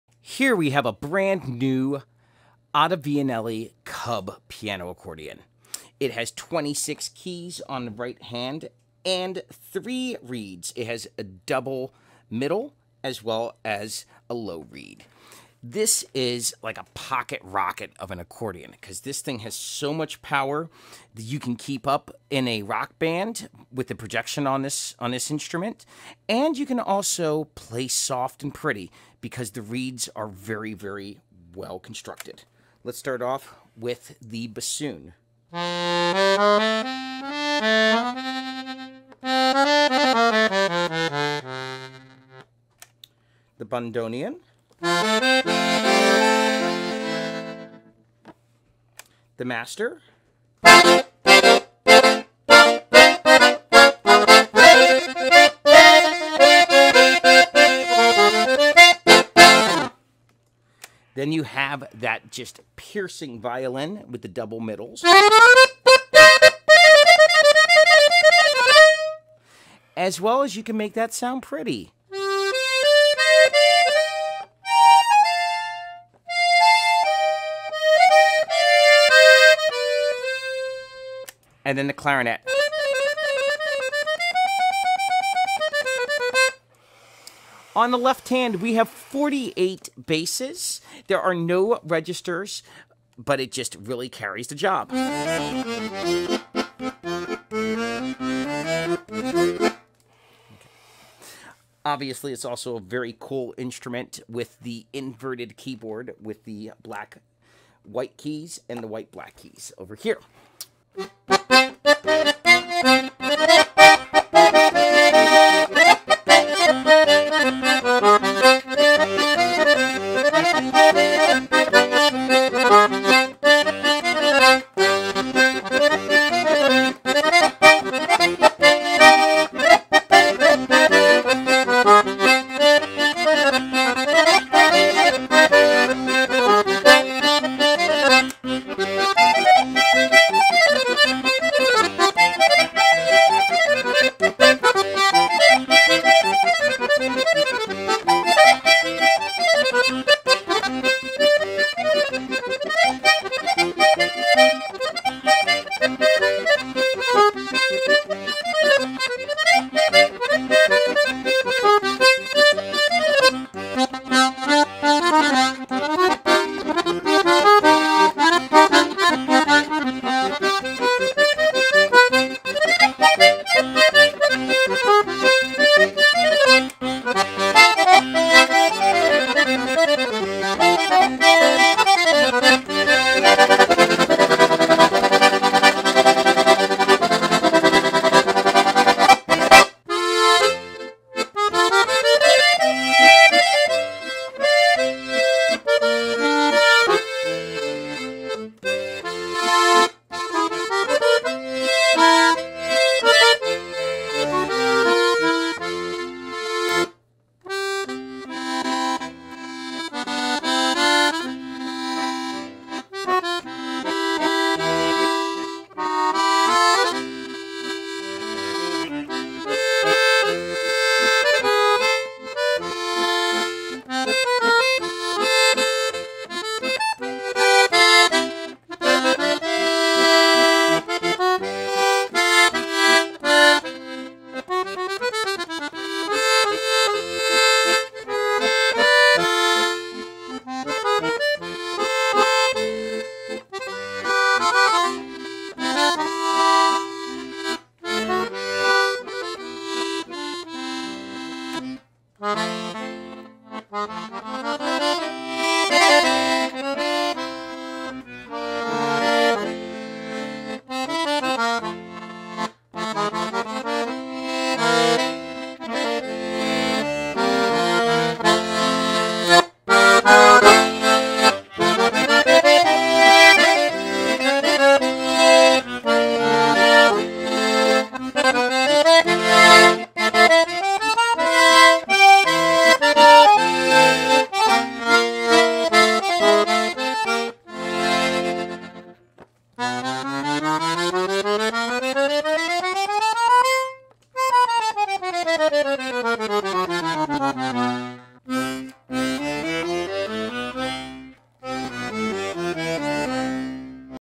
OTTACUBI48BKINV - Black Inverted Ottavianelli Cub I Piano Accordion LMM 26 48 $2999
Brand: Ottavianelli
Model: Cub I
Type: Piano Accordion
Reeds: 3/4 LMM (+5 cents)
Treble: 26 Keys, 12.25"(31cm) Key to Key, 5 Registers
Bass: 48 Bass Buttons, 0 Registers
Weight: 14lb / 6.5kg
Features: Inverted keys, swing tuning, small but mighty!
Includes: New Straps and Case, 3 Year Factory Warranty
Suggested Upgrades: New Hard Case, Professional Microphone Installation, Accordion Stand, Dust Cover, Spray Polish